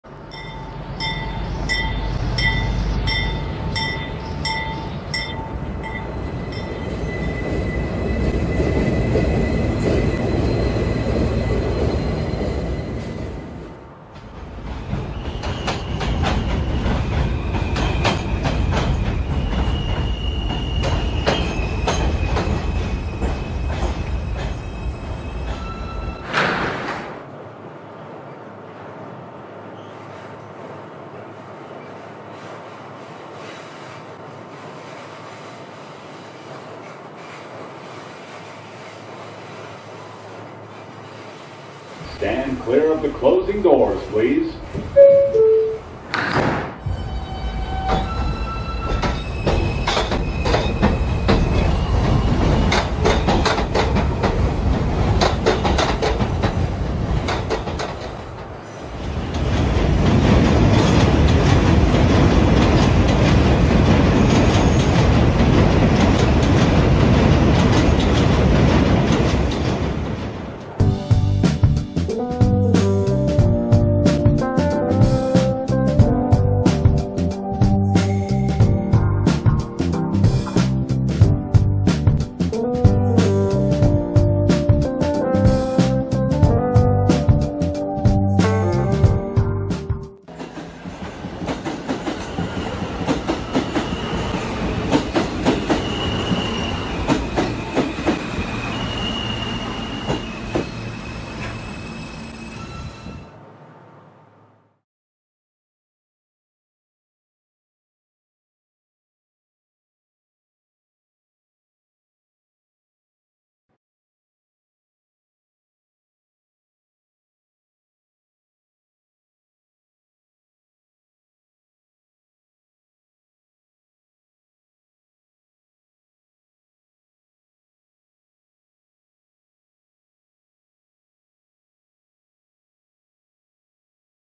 Munipals F Express Train First Run - Subway Mini Clip
It's time to show off the F Express Train's Work Train Delivery and First Ever Test Run on this channel!